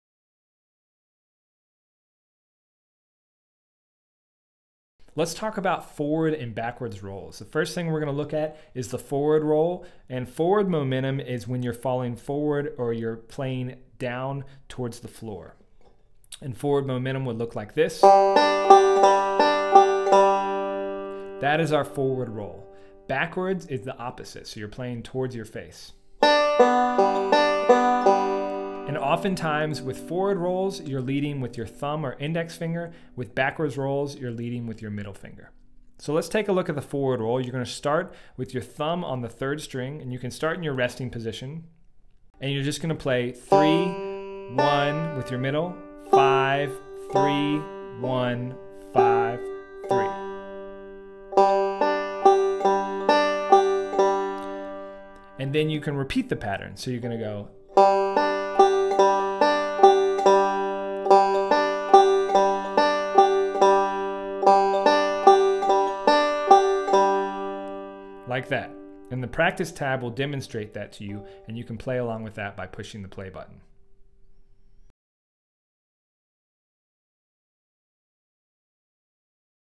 3.1 - The Foward Roll
In this lesson you're going to learn one of the most essential roll patterns for playing Scruggs style melodies and backup - The Forward Roll.

This example uses strings 1, 3, and 5 but forward rolls can use any string combination. The only rule is that your motion is forward, which means your right hand fingers are playing downwards, towards the floor. There are many possibilities of forward rolls and we'll go over a few more examples in future lessons. Remember that when you're picking a string with your fingers you want to come up under the strings and when you play with your thumb you are plucking down.